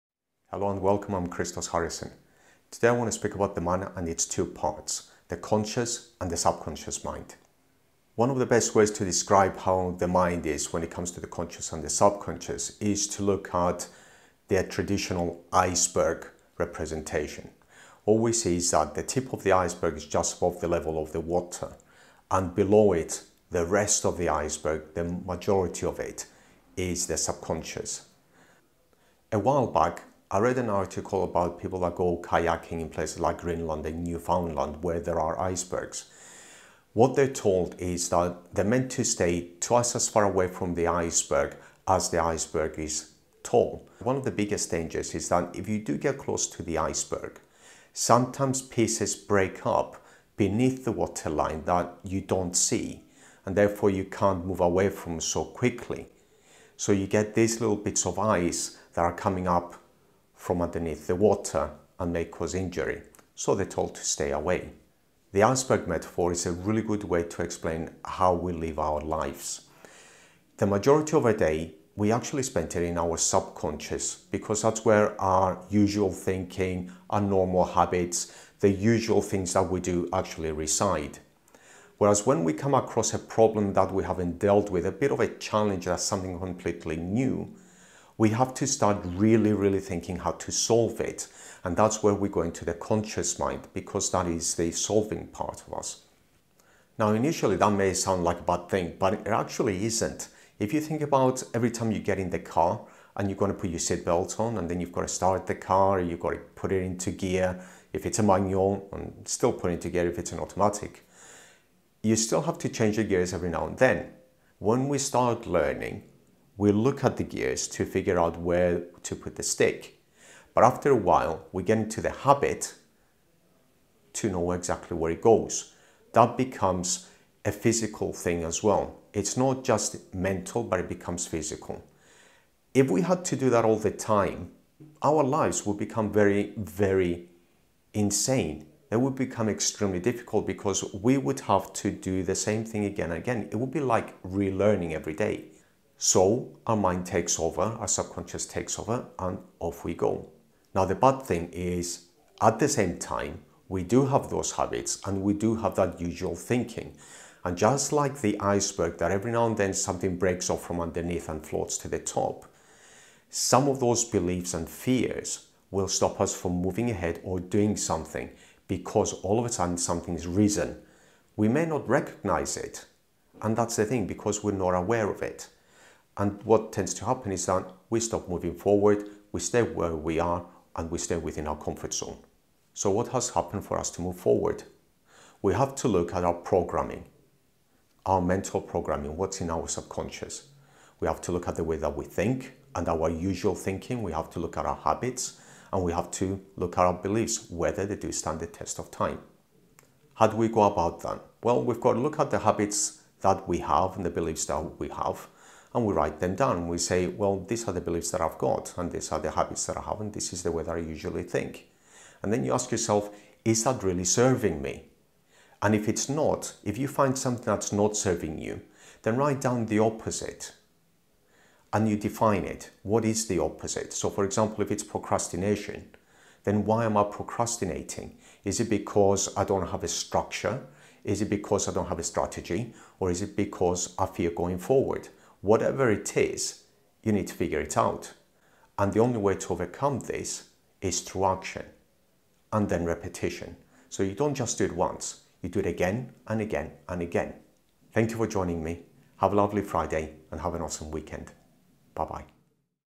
Understanding How Your Mind Works - the Conscious Mind and the Subconscious Mind
The truth is that we can't really separate the mind - it's physically impossible.The mind is intangible.

However, to be able to explain how "it works" we split it into two parts - the Conscious and the Subconscious.

In traditional psychology, it is split into three - the conscious, the subconscious and the unconscious. If we had to go more into psychology, we would have to look at Sigmund Freud, Carl Jung and so on.

To keep things simple, we'll use two terms - the Conscious Mind to describe the part of our mind that we use to logically solve problems that we don't usually deal with and the Subconscious Mind, the part of our mind that is automatic.

It is in the subconscious where all our beliefs, fears and habits reside. To change any of these we have to first become aware of those beliefs and habits that do not serve us, decide to change them and then go about changing them; by replacing them with beliefs and habits that serve us.

To do that we use our conscious mind - by repetition and action.

🔺 Serious and appropriate enquiries only🔺